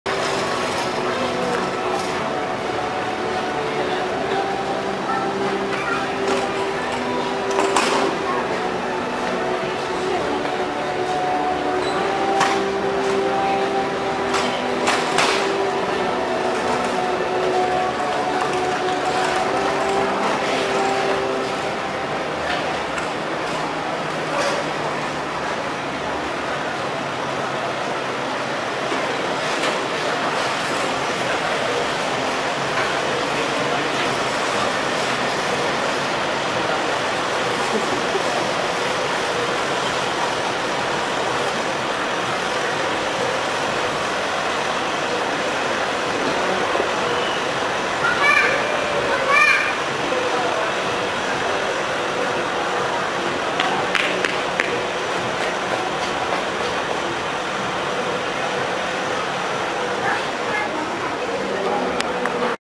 aksi nissan frontier navara di 4x4 challenge giias 2016
video aksi nissan frontier navara double gardan menaklukkan tantangan berupa tanjakan terjal yang memang disiapkan untuk dilahap kendaraan double cabin maupun suv di acara gaikindo Indonesia international auto show 2016 yang bertempat di ICE BSD Serpong Tangerang